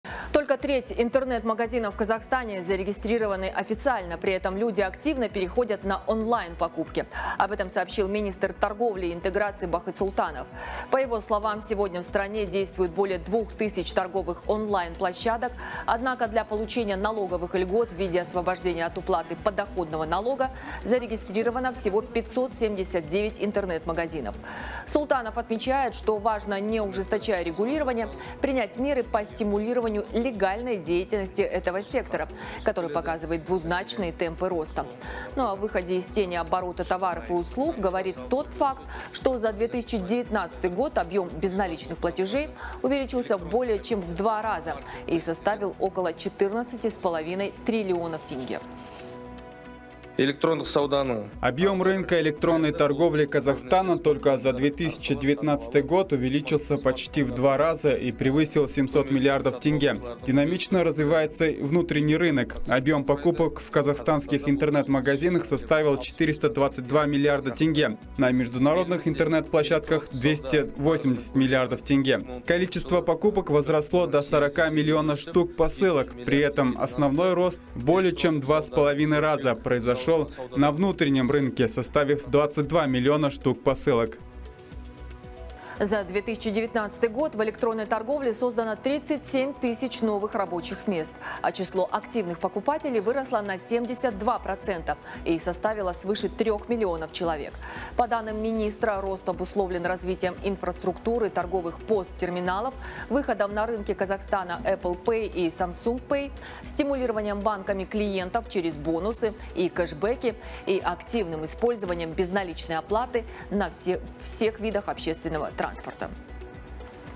КАЗАХСТАНЦЫ АКТИВНО ПЕРЕХОДЯТ В ОНЛАЙН-МАГАЗИНЫ
Только треть интернет-магазинов в Казахстане зарегистрированы официально. При этом люди активно переходят на онлайн покупки. Об этом сообщил министр торговли и интеграции Бахыт Султанов. По его словам сегодня в стране действуют более 2 тысяч торговых онлайн-площадок. Однако для получения налоговых льгот в виде освобождения от уплаты подоходного налога зарегистрировано всего 579 интернет-магазинов. Султанов отмечает, что важно, не ужесточая регулирование, принять меры по стимулированию легальной деятельности этого сектора, который показывает двузначные темпы роста. Об «обелении» оборота товаров и услуг говорит тот факт, что за 2019 год объем безналичных платежей увеличился в более чем 2 раза и составил около 14,5 трлн тенге.